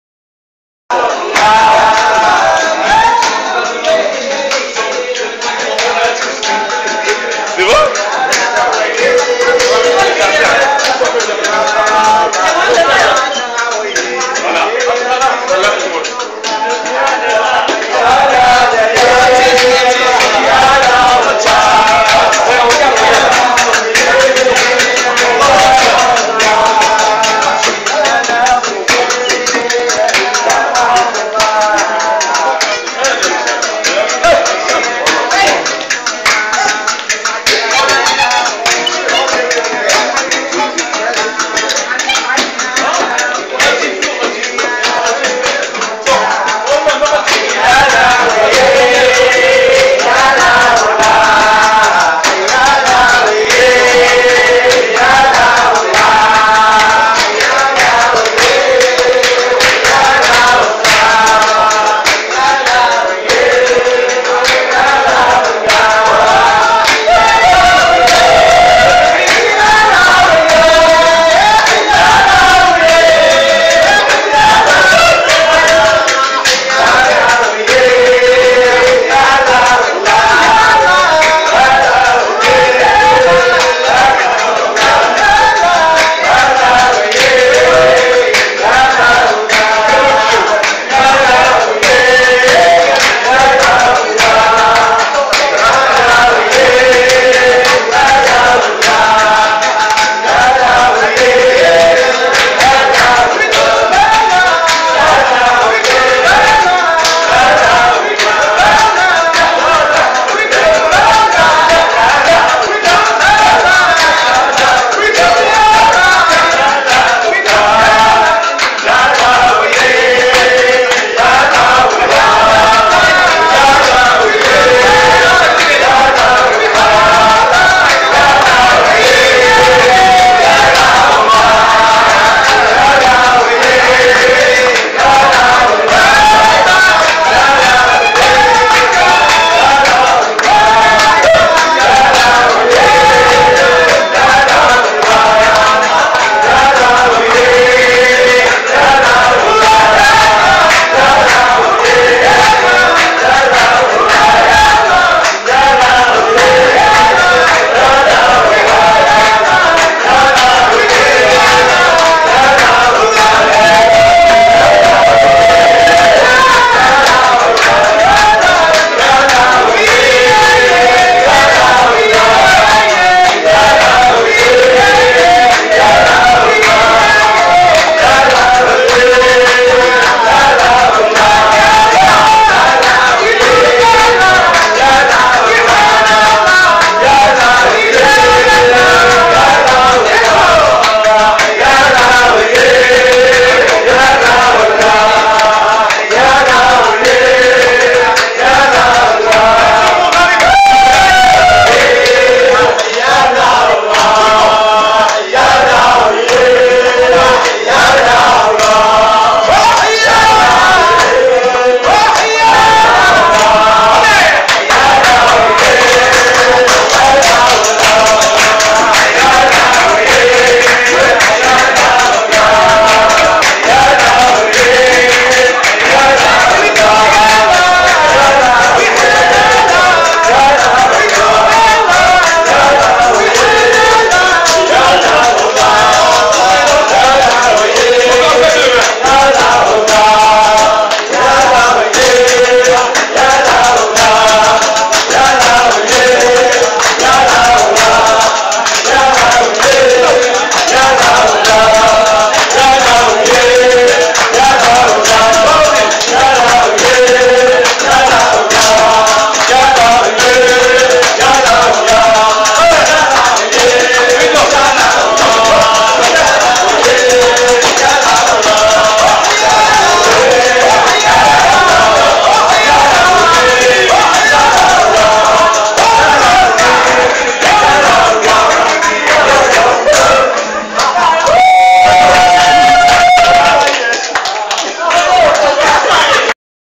ffcak fete golden tulup 2014 06 19 23 57 28communpress media
اختفال الضيوف الأفارقة داخل فندق فرح توليب خلال الدورة 17 من مهرجان السينما الافريقية ما بين 14 و21 يونيو 2014